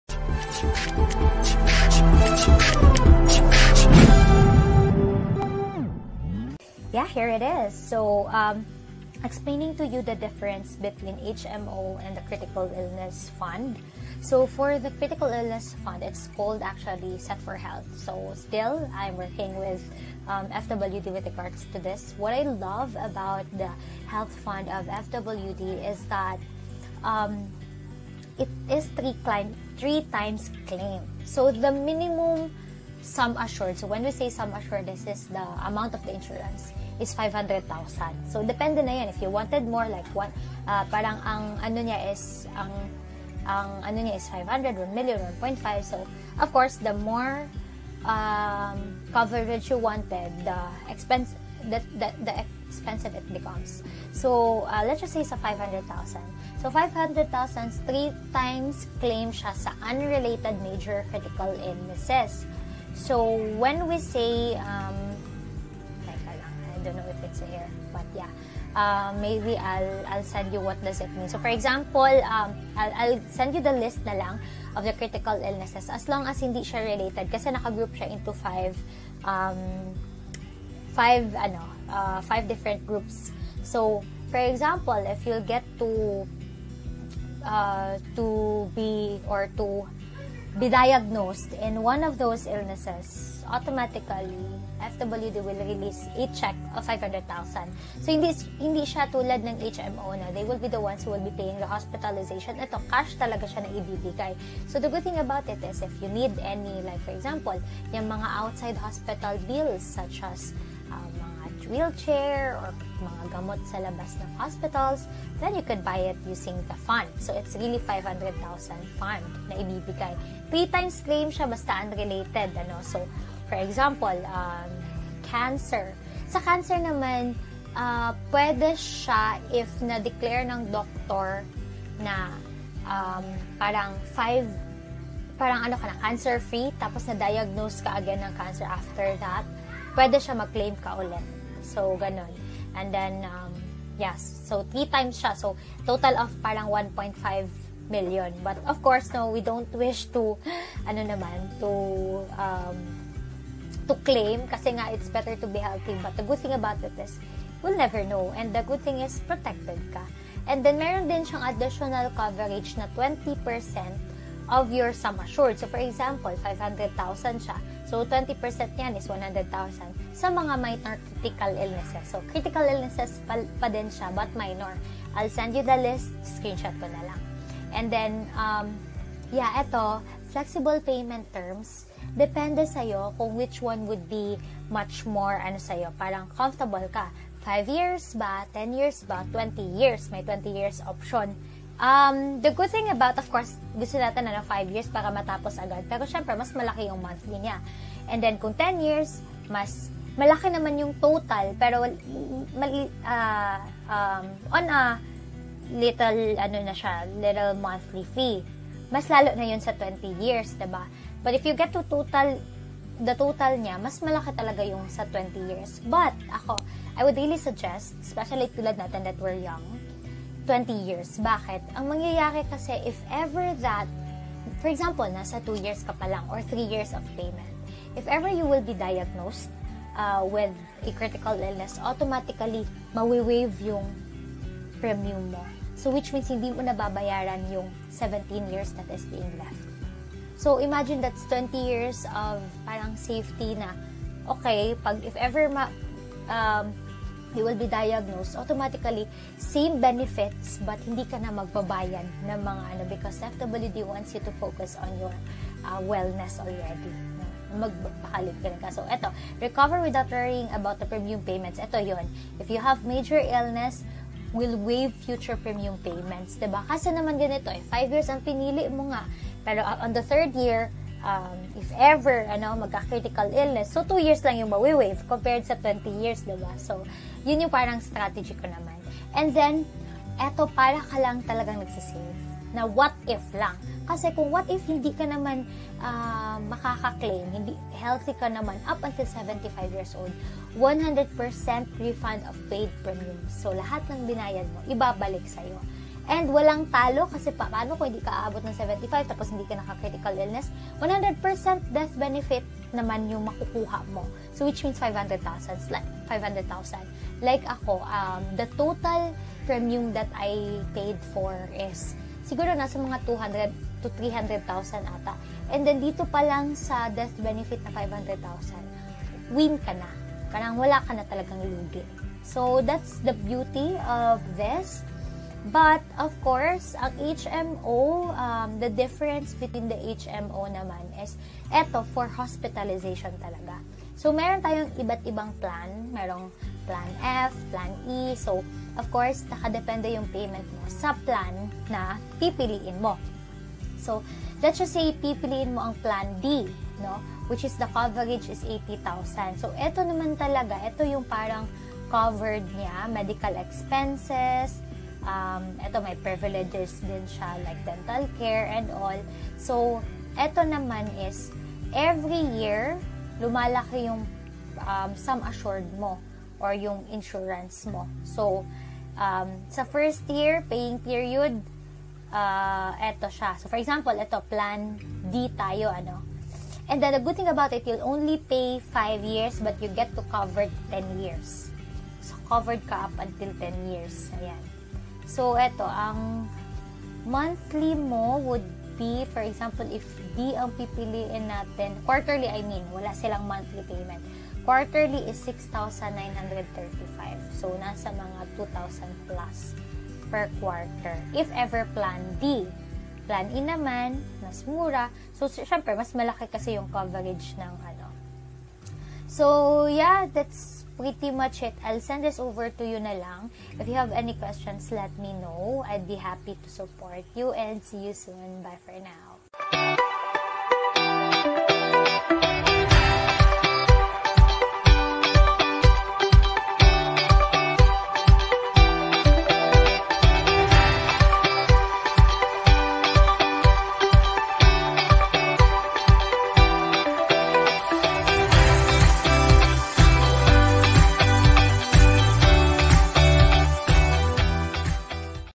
Difference of Health Fund Critical Illness and HMO
BAWAL MAGKASAKIT!

Everyone agrees that this phrase is uttered quite often, and I'm sure it's clearly intentional.

Take a look at the treatment packages offered by the Philippine Heart Center, and you will see that some of these procedures can cost anywhere from several hundred thousand to more than a million Philippine pesos.

Because we can't afford to get sick today, we can truly say "Bawal magkasakit"!

And that's where your INSURANCE kicks in! Don't underestimate the importance of financial planning because it allows us to be better prepared for life's unexpected events, such as sickness and critical illness.

Check out this video to learn about the differences between HMO and FWD Critical Illness Funds, and how you might benefit from either of these options.